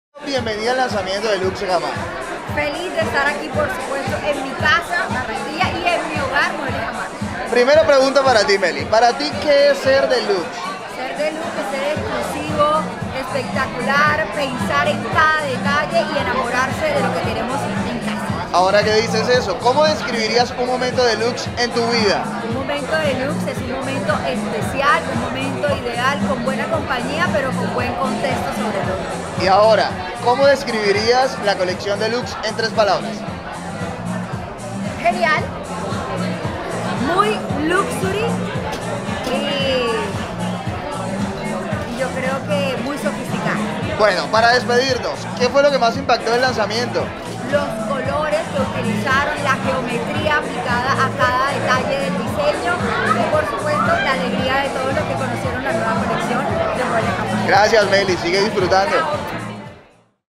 Melissa Martínez | Paparazzi Jamar | Lanzamiento Deluxe ✨
Recordemos el lanzamiento de la colección Deluxe, fue una noche de elegancia, sofisticación y lujo fue lo que se vivió en el lanzamiento de nuestra nueva colección Deluxe, inspirada en espacios modernos con acentos delicados y detalles geométricos que elevan la visión del estilo en el hogar. Los famosos cayeron en el lente de nuestro Paparazzi Jamar y respondieron todo acerca de la nueva colección.